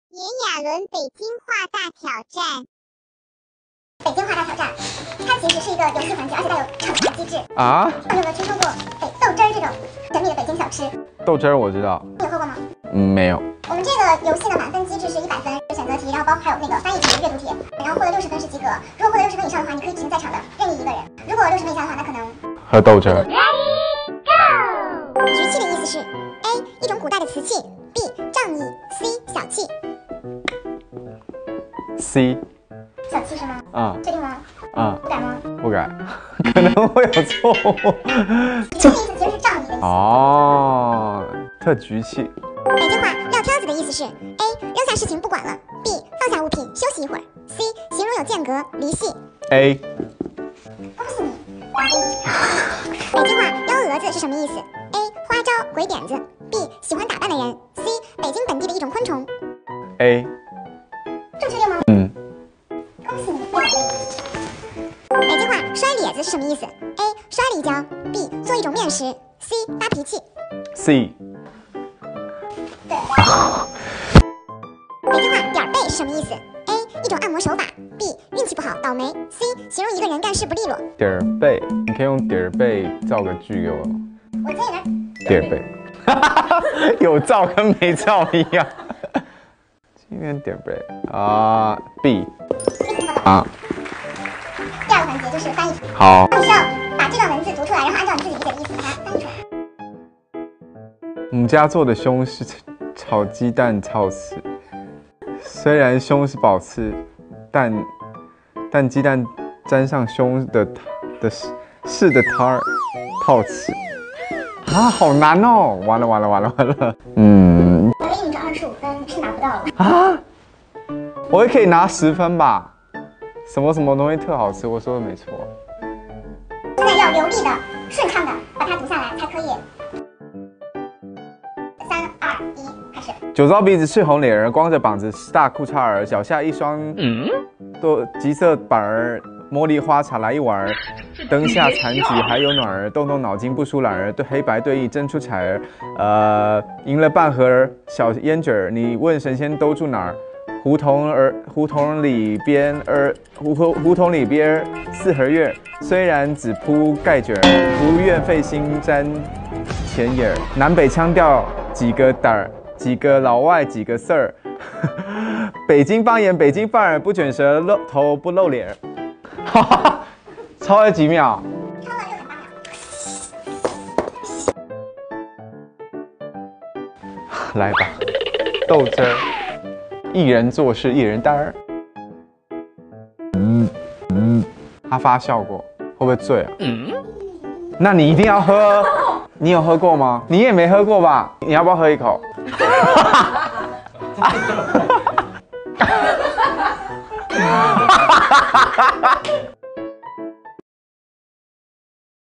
20200121【Rayli 瑞麗網】炎亞綸北京話大挑戰來啦！
這次瑞醬出了不少難題，考驗亞綸對北京話的理解是否正確？炎亞綸說兒化音也是超級有趣的哦！快來點擊視頻，康康亞綸最後得了多少分吧！